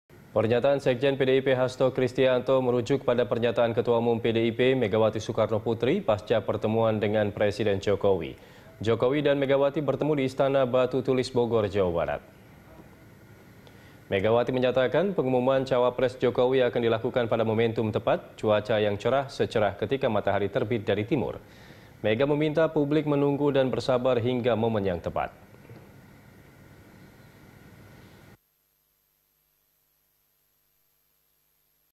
Pertemuan Jokowi dengan Mega Bahas Nama Cawapres
Nama cawapres disebut menjadi pembahasan dalam pertemuan Presiden Jokowi dengan Ketua Umum PDIP Megawati Soekarnoputri.

Facebook.com/BeritaSatuTV
Youtube.com/BeritaSatu